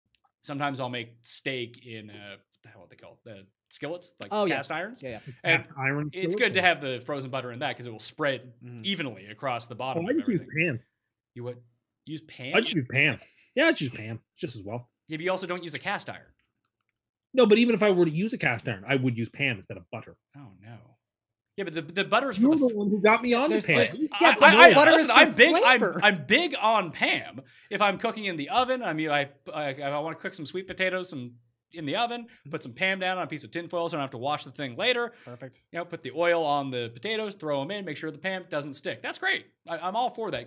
Cooking STEAK with PAM - PART 1
You read that correctly. Enjoy this multi-part Short where we discover Tim has been cooking steak with pam.